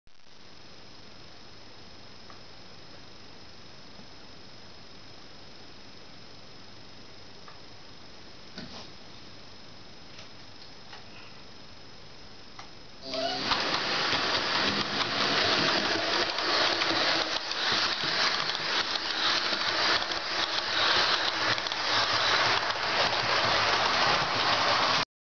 Roco BR 101 (62339) mit Doppelstockwagen
BR 101 von Roco mit 3 Fleischmann Doppelstockwagen die Beleuchtet sind beim Losfahren